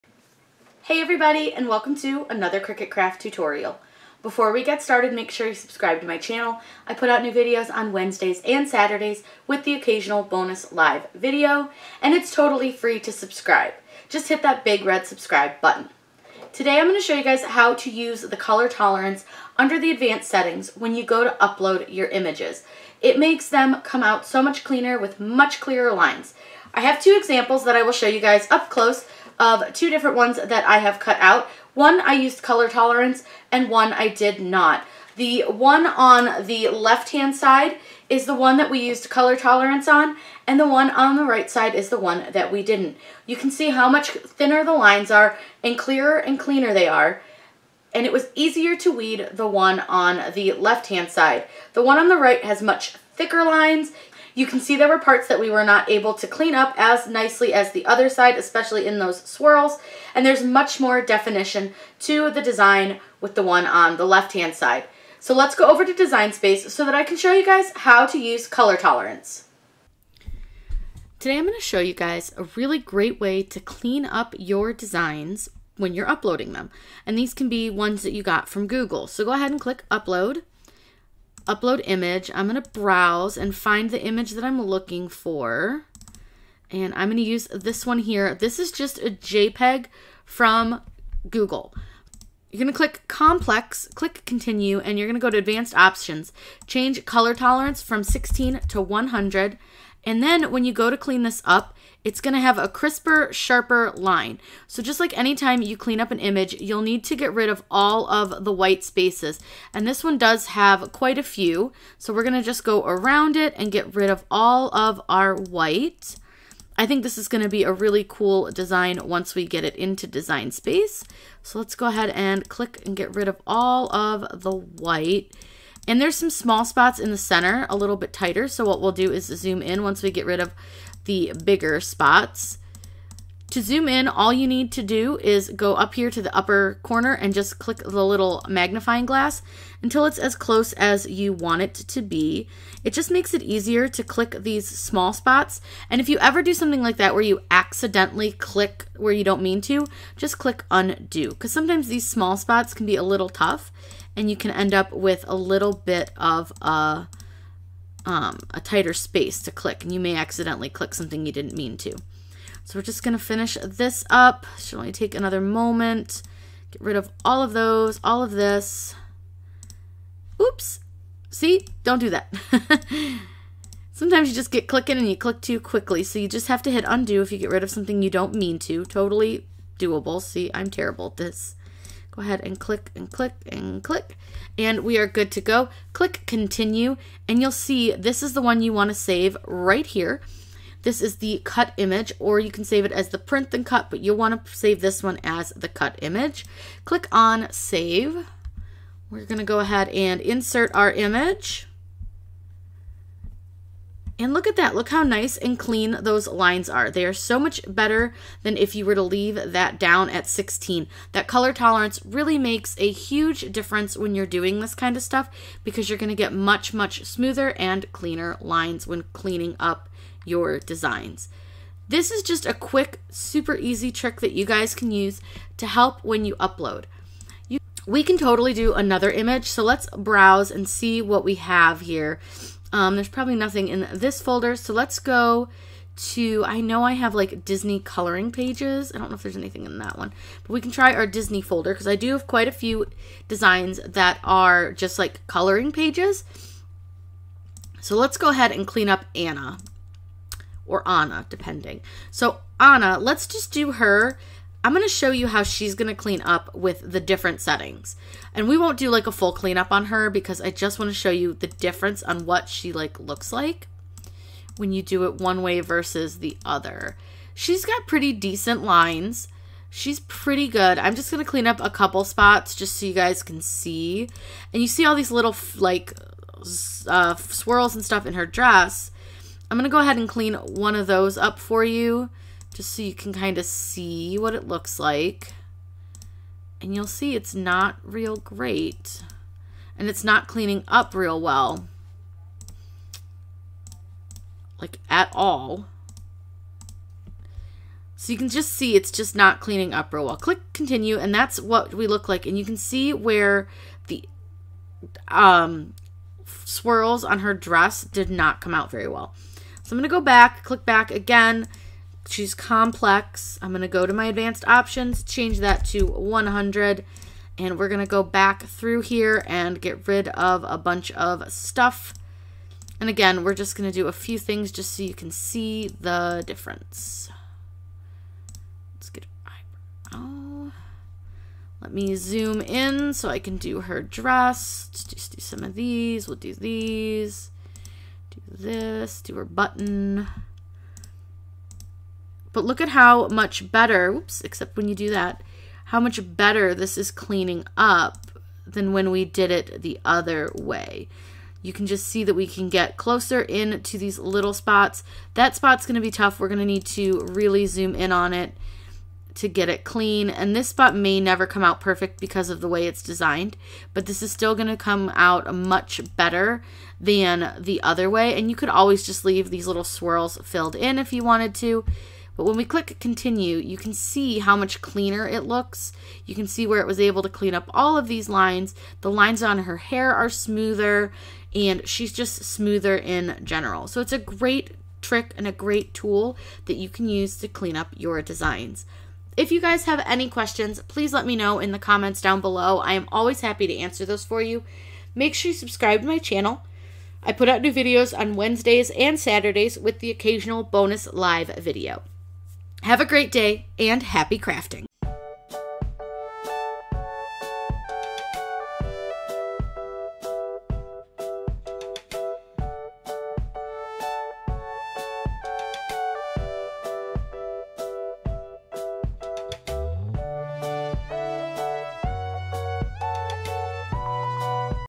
How to upload images in cricut design space using color tolerance for a clean smooth image
Using complex and color tolerance will really give you a much cleaner clearer image to cut with. The lines are sharp and crisp!
Be sure to tag me on Instagram on your photo if you make this project!  @corinne_Blackstonecrafts

JOIN ME ON MY OTHER SOCIAL MEDIA PAGES